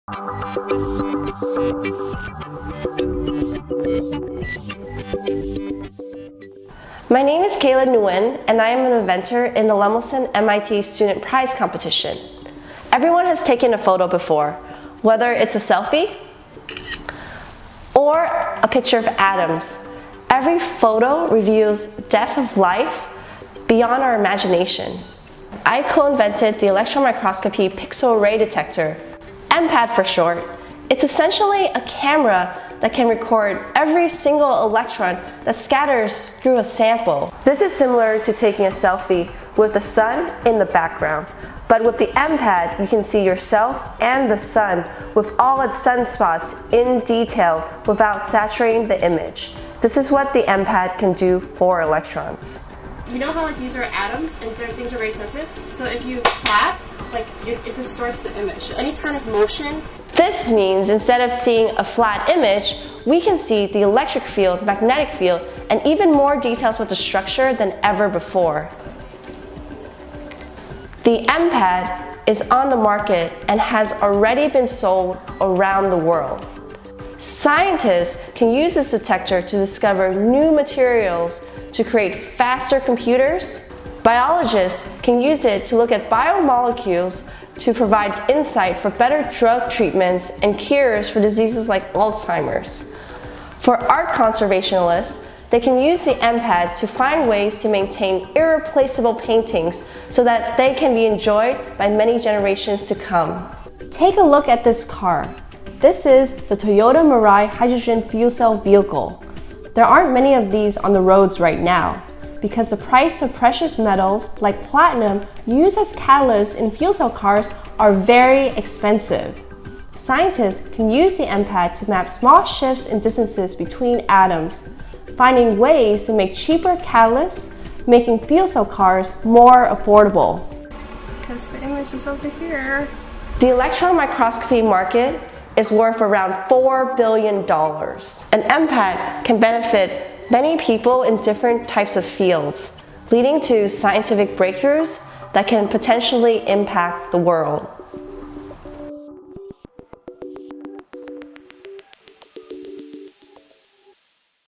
Kayla Nguyen: $15,000 “Use it!” Lemelson-MIT Student Prize Graduate Winner
EMPAD is a highly-efficient detector for the electron microscope which she developed with a group of scientists from the Muller and Gruner research groups. The detector is a special camera designed to detect and display electrons at a much greater level of detail than existing versions. Her secondary invention, the airSTEM, is a high-performance and low-cost scanning transmission electron microscopy detector. Kayla earned the $15,000 2018 “Use it!” Lemelson-MIT Student Prize for her inventive work.